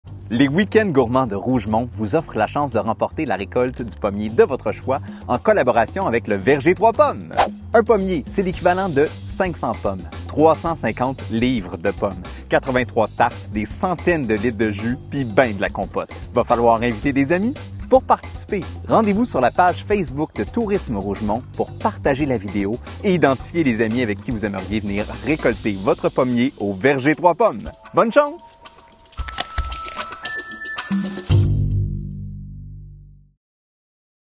Concours - Gagnez un pommier 2020
Présentation du concours « Gagnez un pommier » en collaboration avec le Verger Trois pommes.